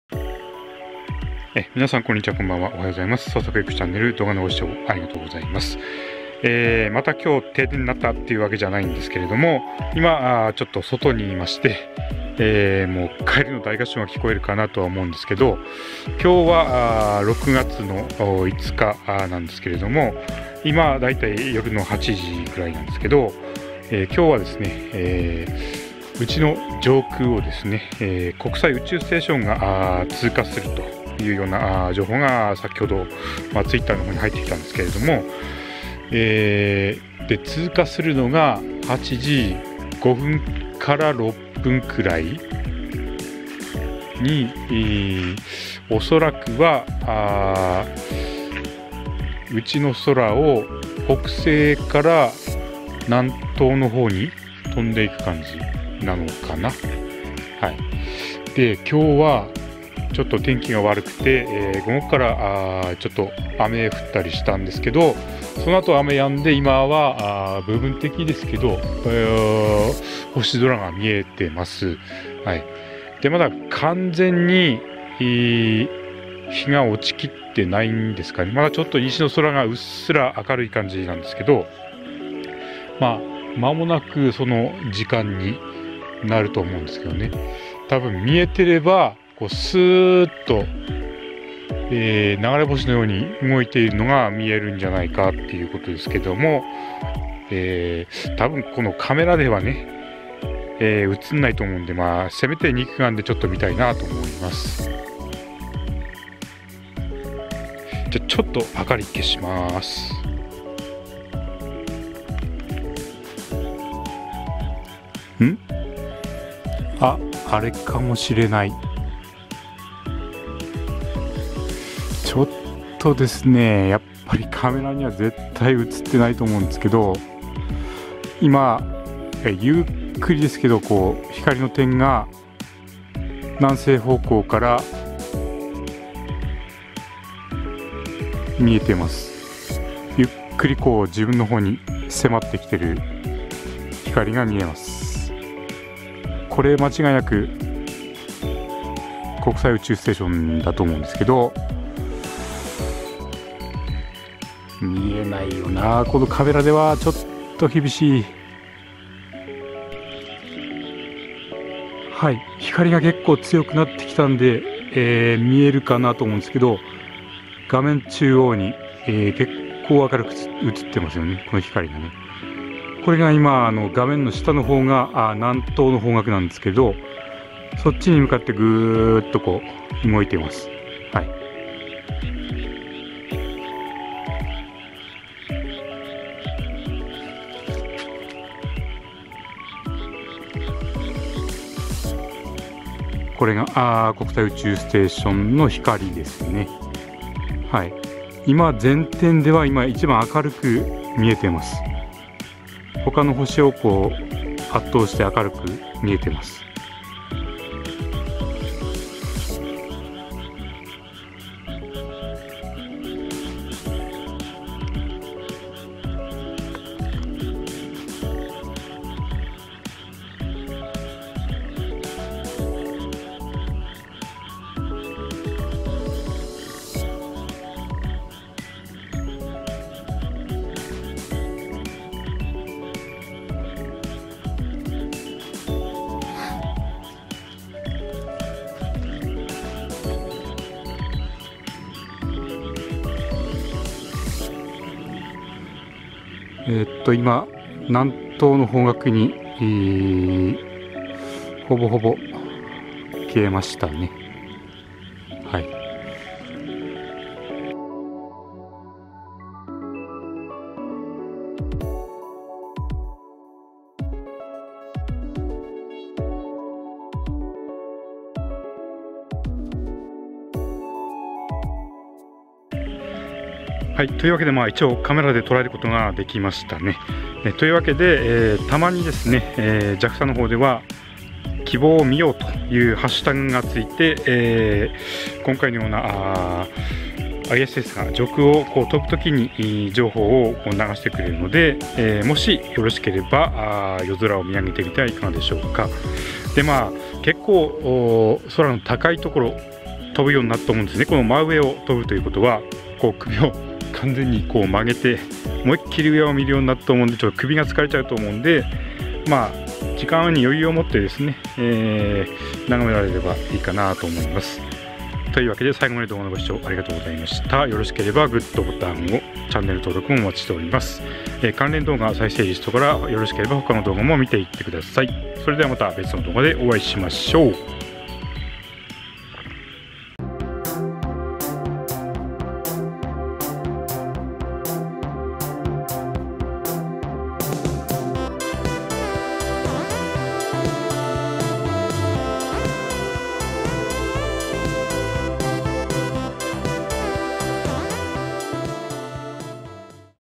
きぼうを見よう Look at the ISS with the hashtag ”Let's see Kibo”
JAXA宇宙ステーション・きぼう 甲方・情報センター
「きぼう」/ISSの目視予想情報

6月5日はちょうどよく福島の真上を国際宇宙ステーションが
通過するとのことで雨上がりの庭に出てみました!
方角的には北西の方角から南東に向かって移動するはずと
待ち構えていました。自分のコンデジ（Canon G7X Mark II）
では到底映らないと思っていましたが、静止画・動画ともに
何とか収めることができました！

BGM・SE
YouTubeオーディオライブラリー